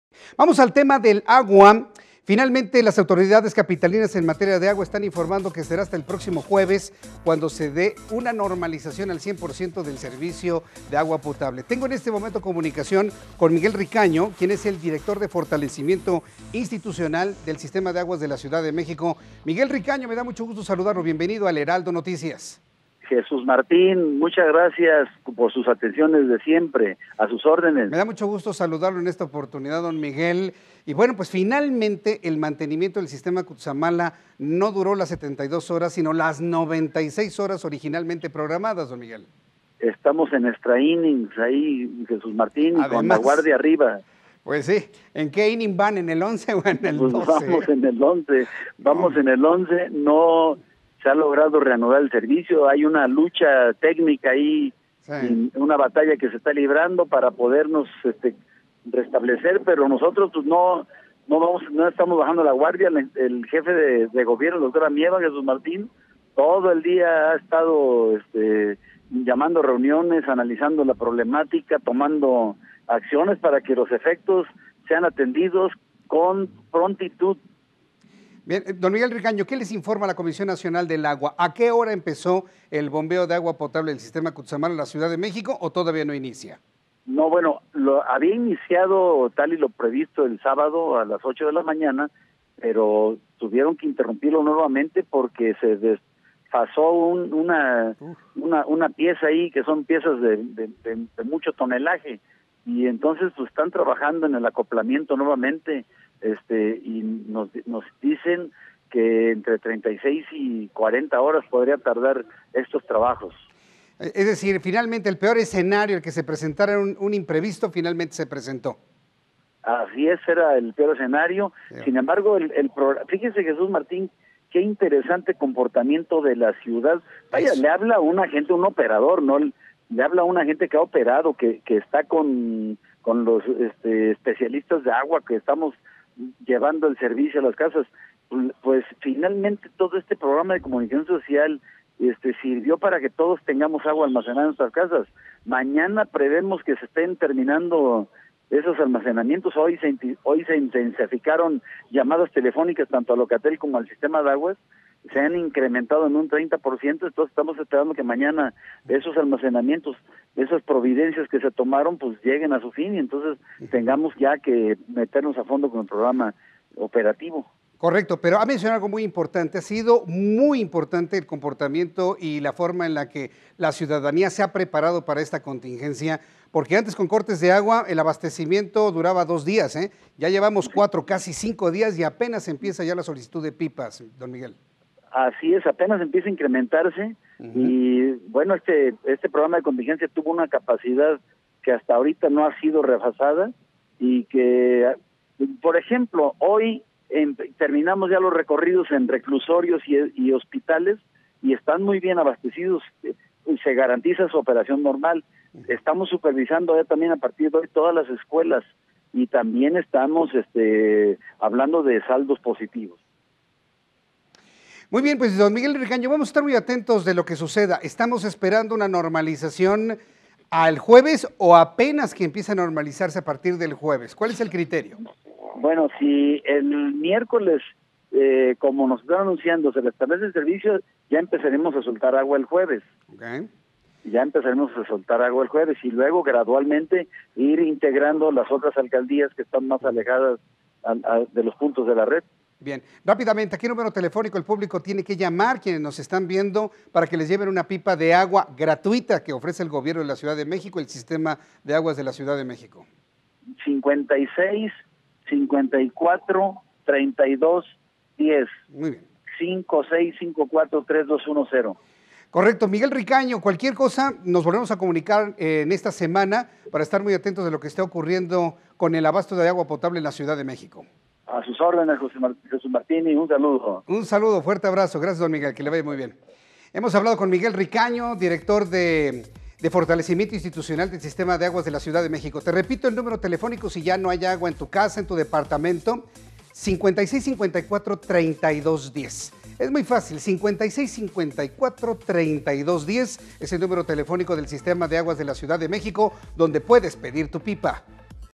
Mantenimiento en el Cutzamala se prolongará hasta el jueves
Jesús Martín Mendoza platicó en exclusiva con Miguel Ricaño, director de fortalecimiento institucional del Sistema de Aguas de la CDMX, para hablar sobre el megacorte de agua